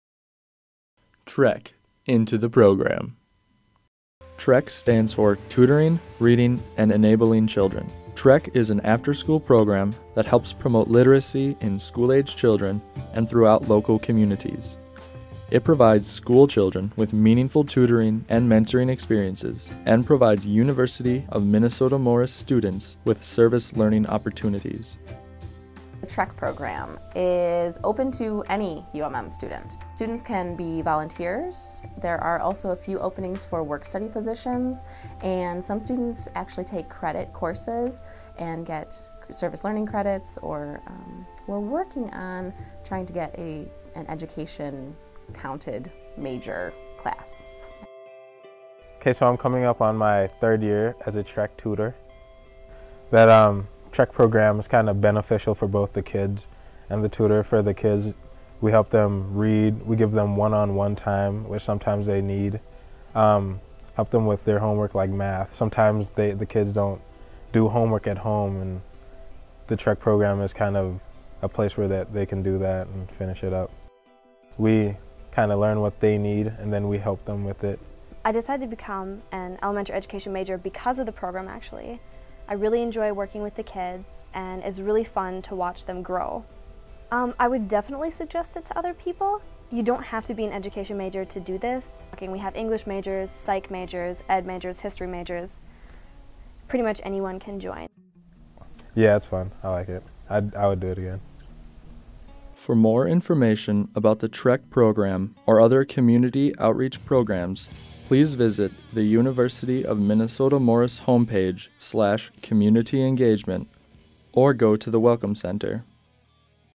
Tutoring Reading Enabling Children at the University of Minnesota, Morris
TREC into the Program

University of Minnesota, Morris students volunteer as tutors for local children, helping with math and reading homework as well as giving children much needed one on one time.